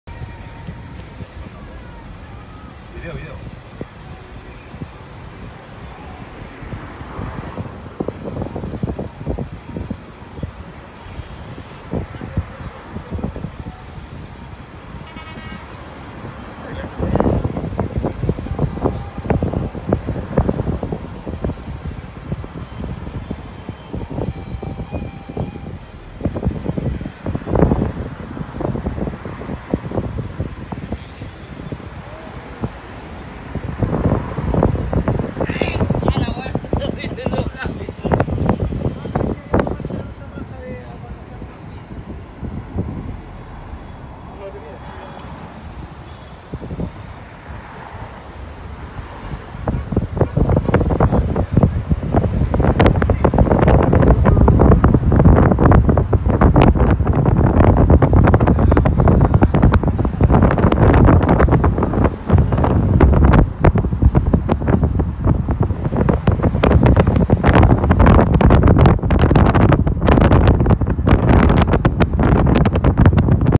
Camioneros bloquean el camino La Pólvora en Valparaíso por demandas de seguridad
Todo esto tras la muerte de su colega Juan Barrios en Victoria. Son cerca de 60 máquinas.